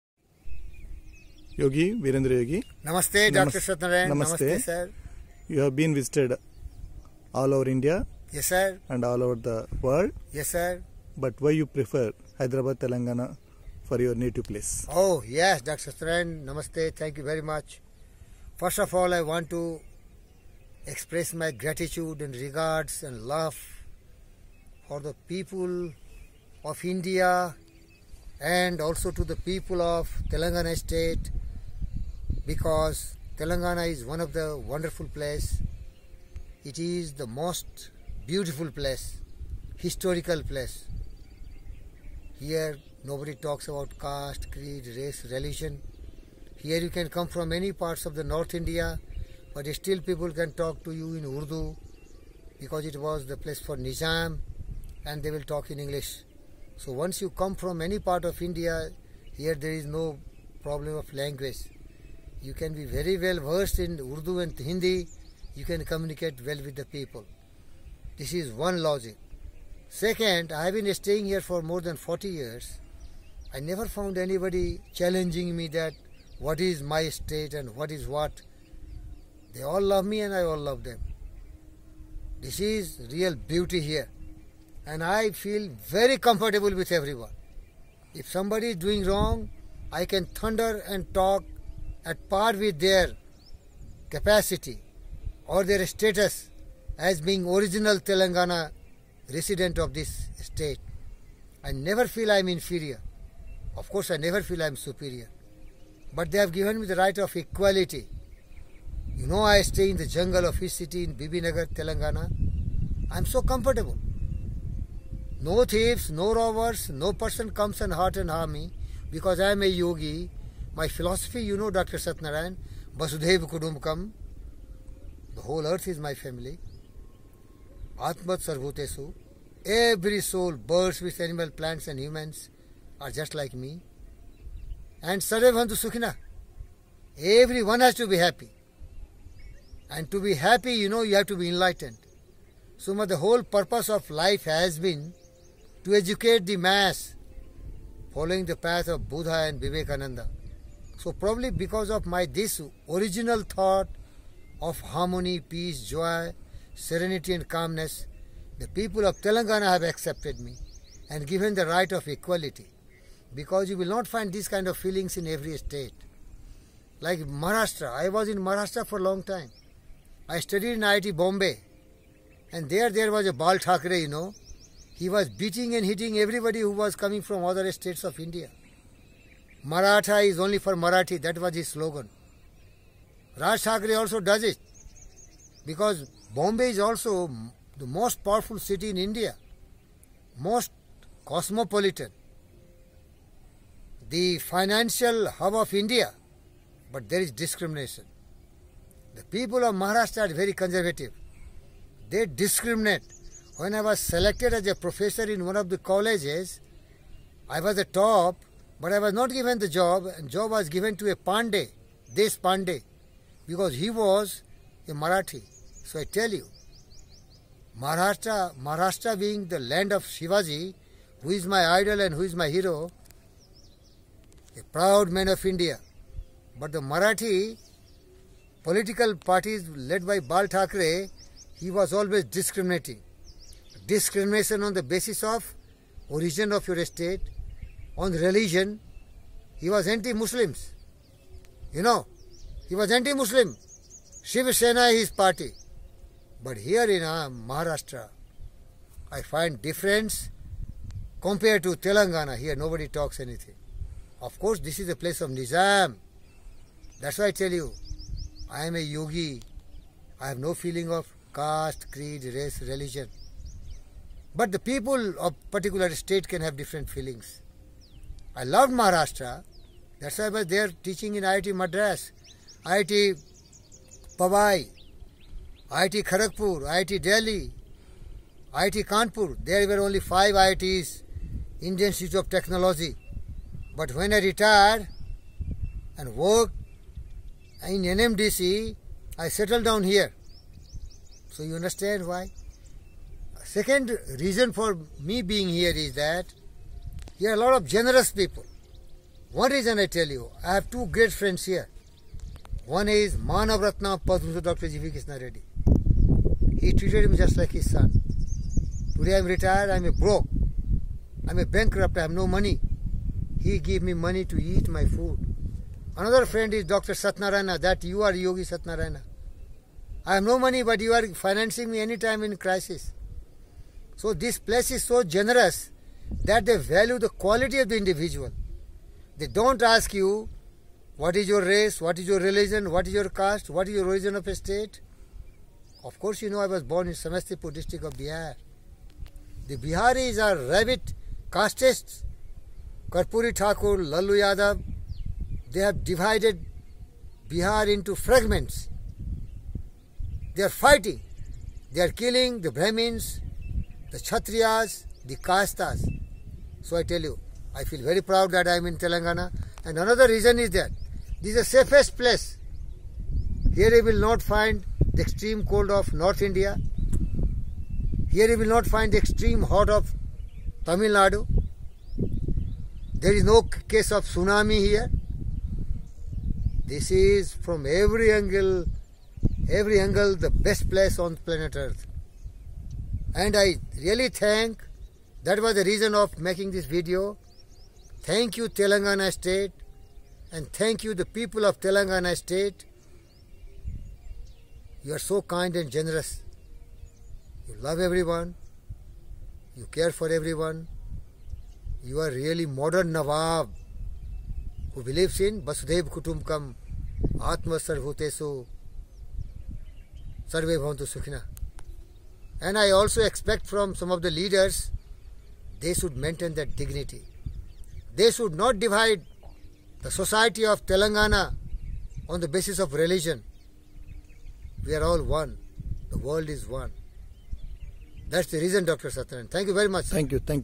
City Of Hyderabad,TS,India ..Unparalleled..love for all,harmony and shelter for anybody
Hyderabad has its fair share of marvels to offer to tourists and travellers. This city is the perfect blend of busy crowds, old traditions and cultures, amazing food and spectacular sights. Undoubtedly, these are the reasons why Hyderabad is often known as the New York of India. This city is the perfect example of how modern day living and culture go hand in hand with age-old traditions and lifestyle